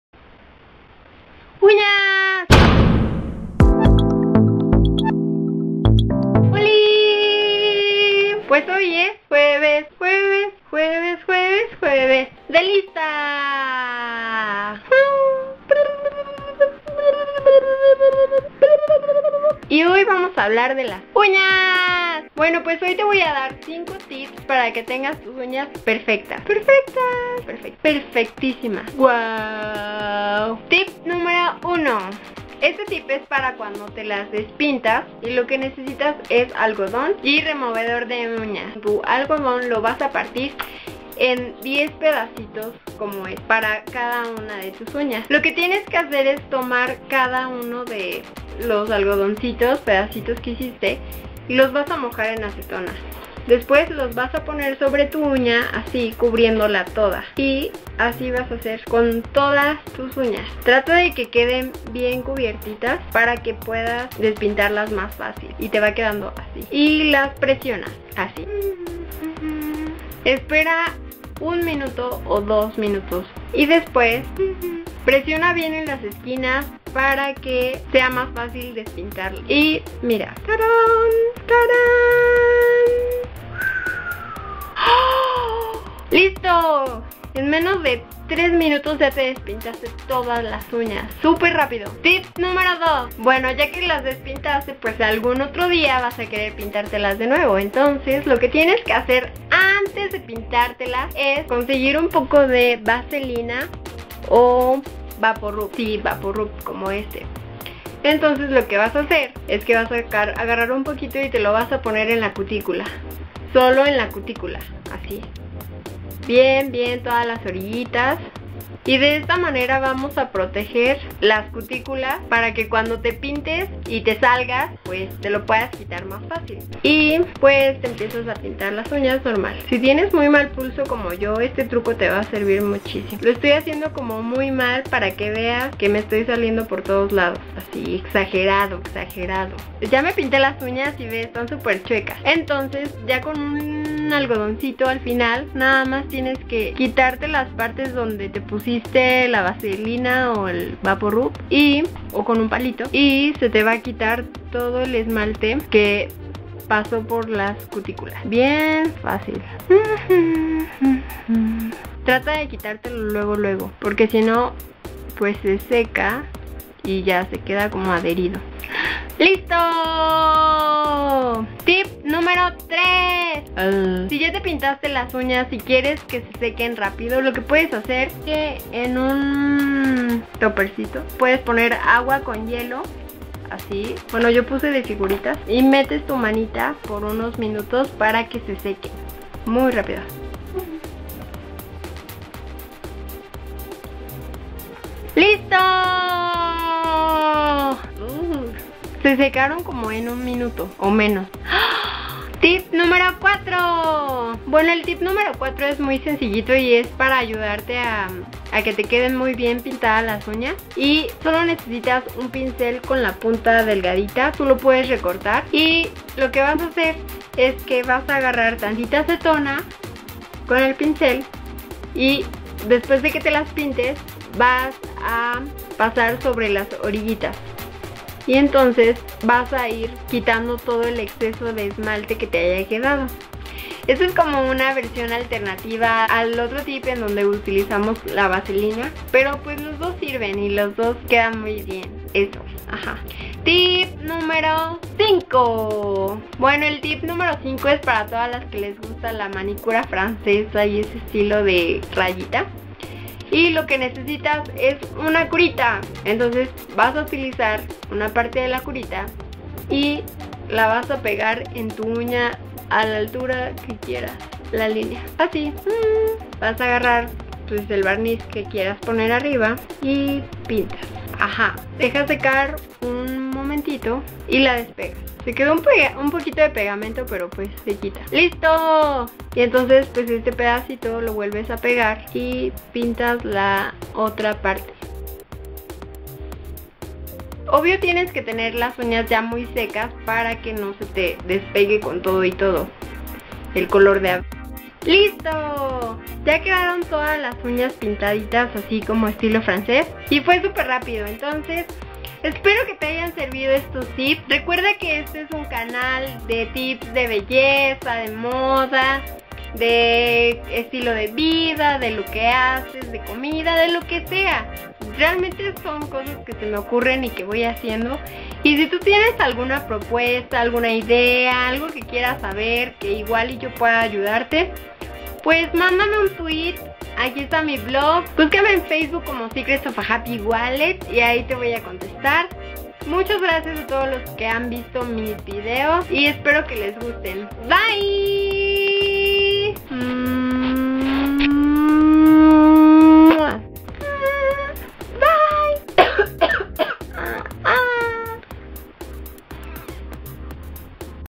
5 tips para unas uñas perfectas!!
Holi!! Hoy es jueves de lista!! y hoy les dejo 5 tips super fáciles para que sus uñas queden hermosas en muy poquito tiempo! :D Espero les sirva bye!!!